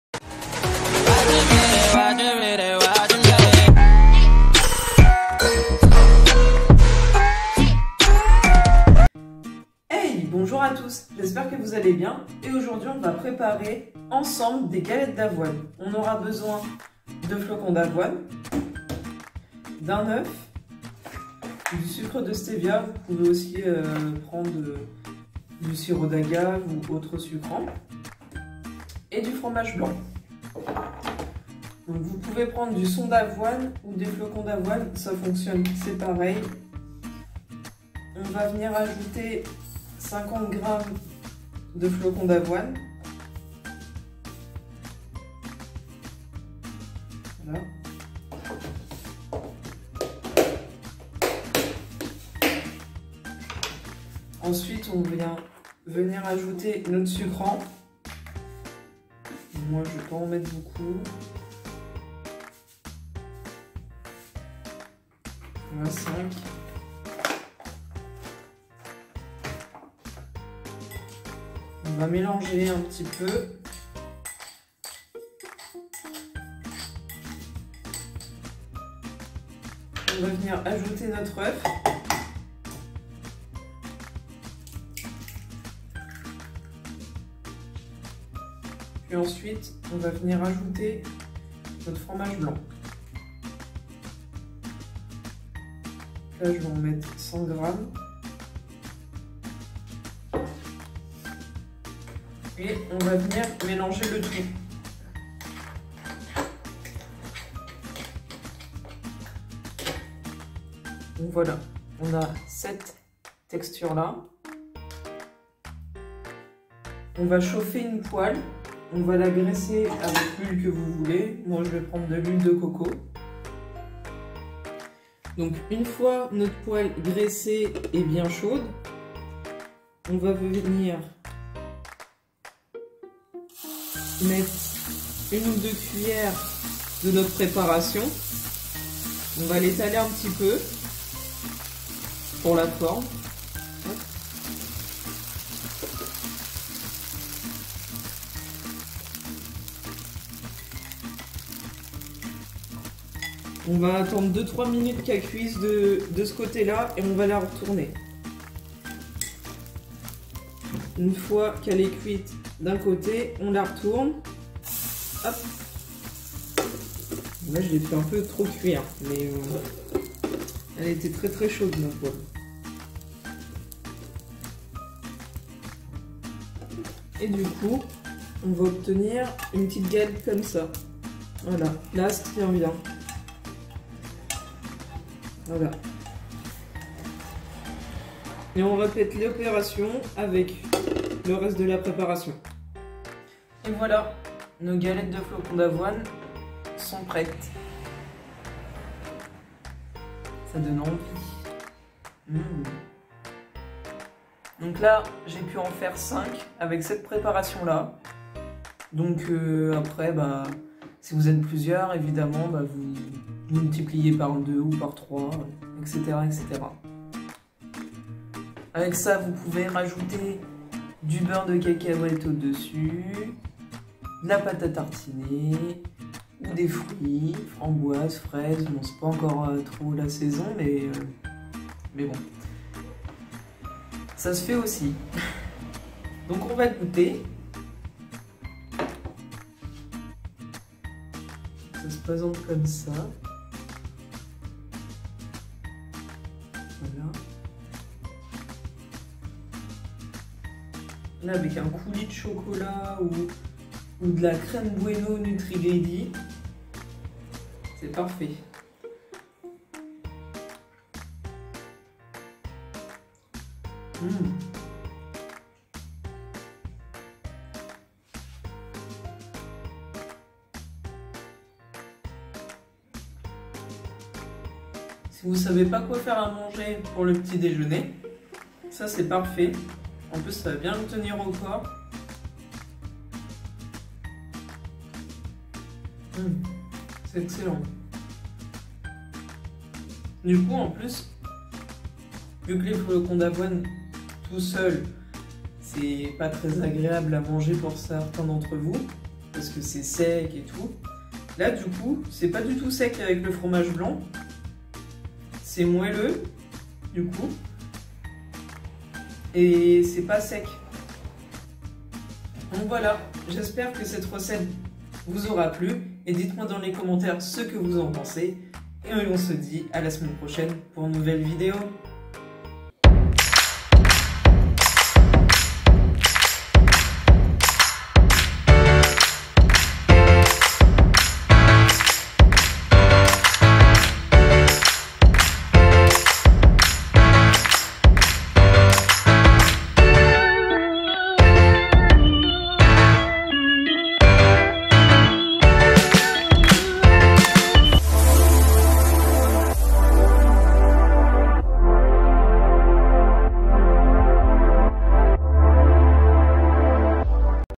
Galettes de flocons d'avoine: recette simple et facile !
Ingrédients pour 5 galettes :
50g de flocons d'avoine
1 œuf
100g de fromage blanc
5g de sucre de stévia

Valeur nutritionnelle pour l'ensemble de la recette :
304 calories
35.4 g de Glucides
19.5g de Protéines
9g de Lipides

Pour 1 galette
60 calories
7.8g de Glucides
3.9g de Protéines
1.8g de Lipides

Dis-moi en commentaire si tu veux que je fasse une recette en particulier !